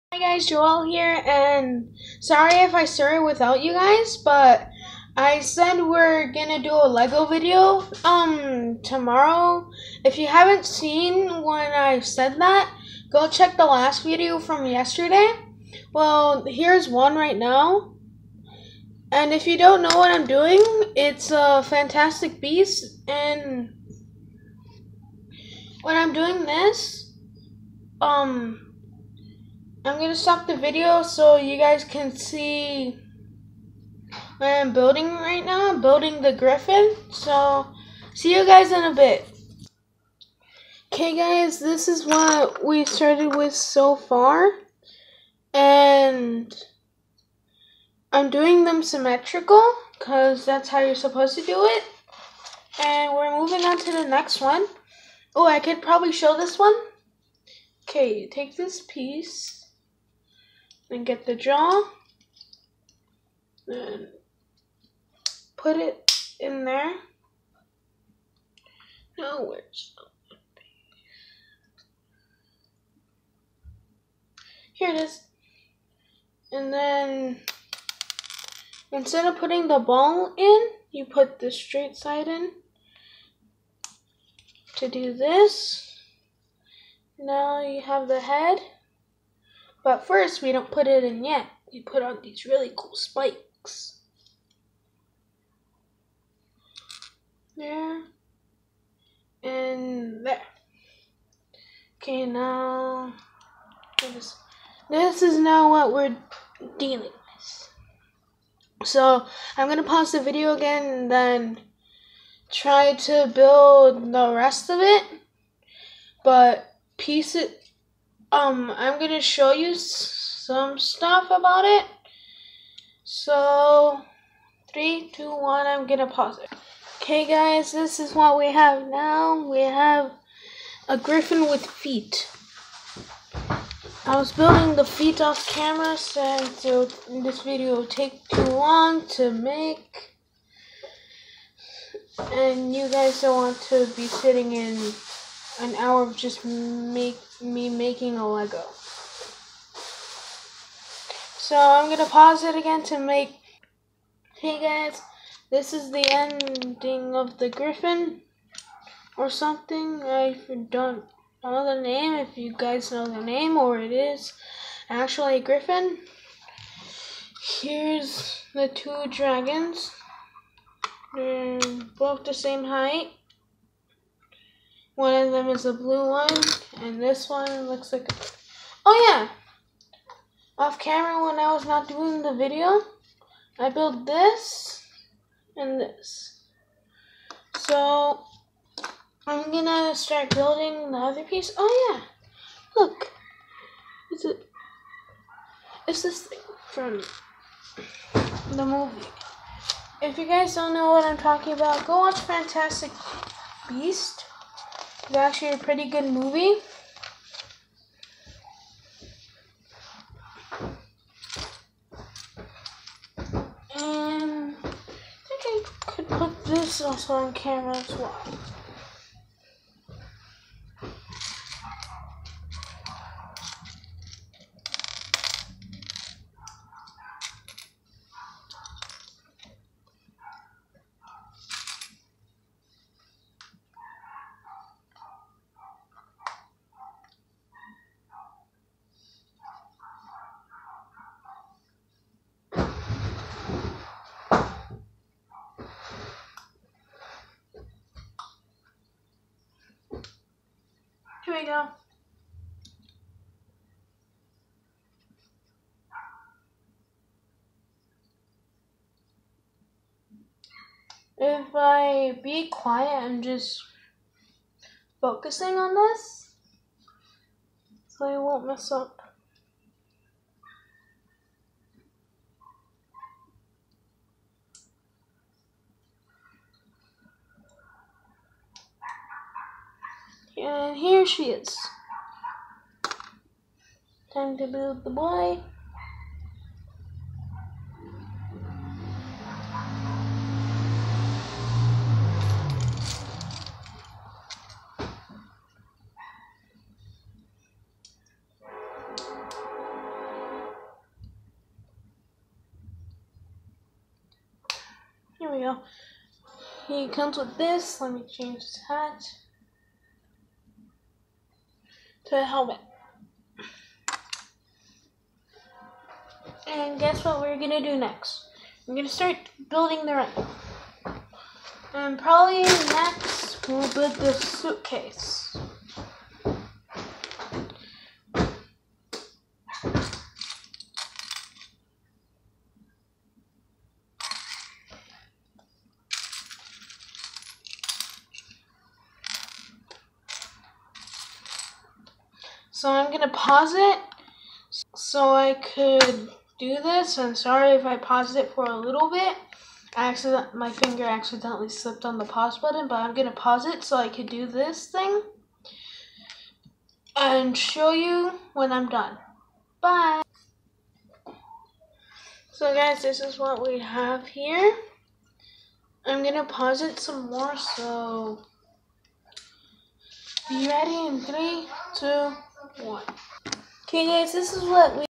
Building the fantastic beast Lego set
This is a review of a Lego set i bought at walmart i will do a 2nd part to show you the finish set